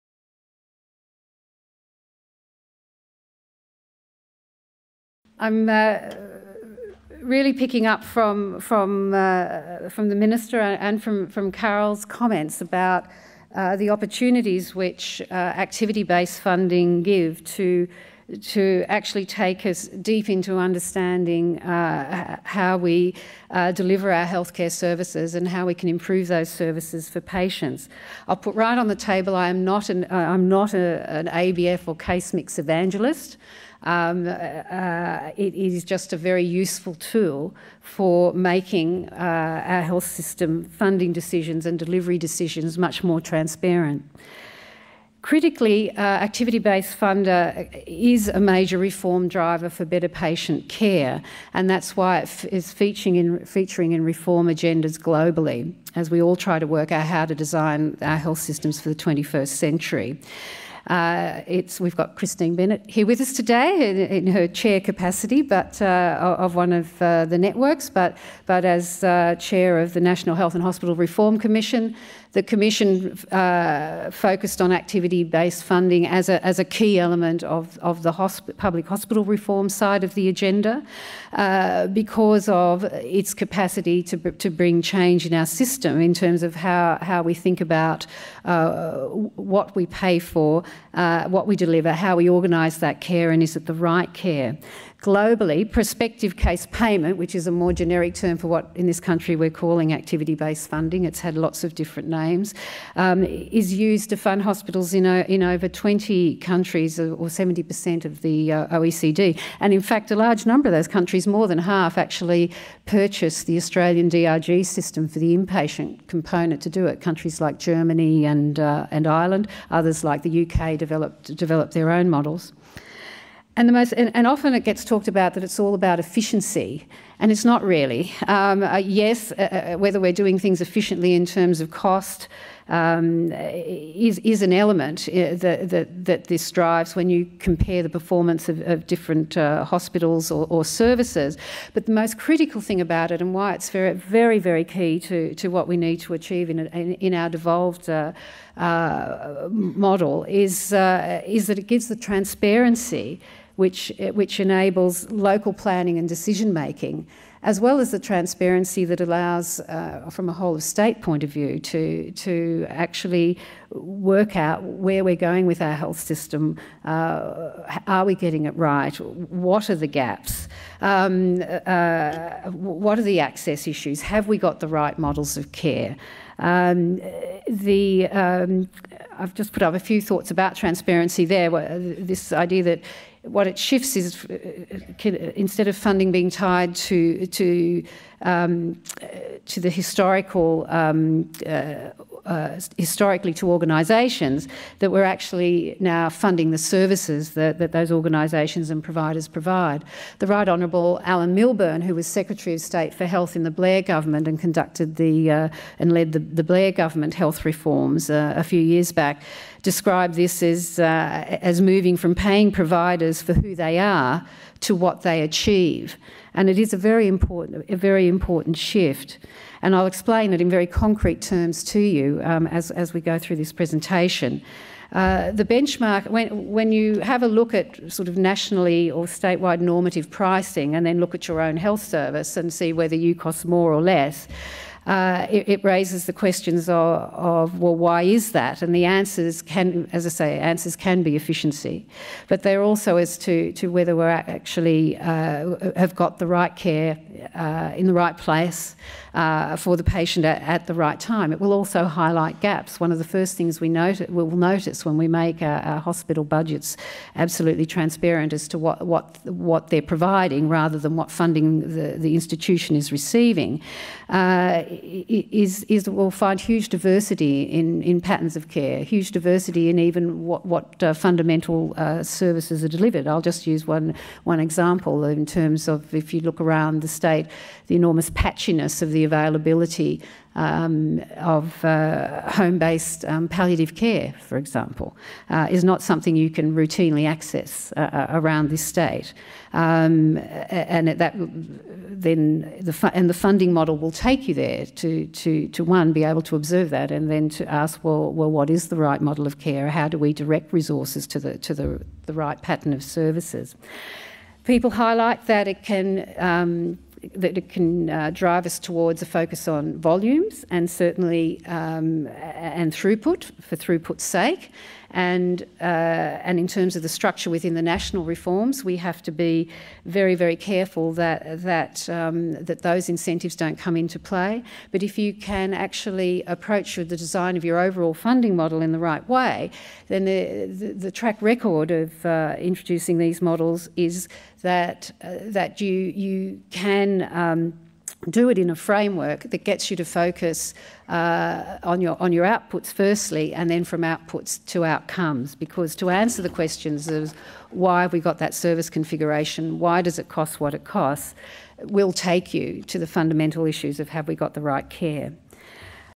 Director General Mary Foley - Health Care Services
A health care system to meet our needs. The Next Step: Funding Reform was the focus of this state wide Symposium for clinicians, community representatives, local health managers, and State and Commonwealth health administrators.
Comprehensive information about delivery on commitments to NSW clinicians and community in reform of the health system and progress on the new funding model was provided with wide discussion among participants.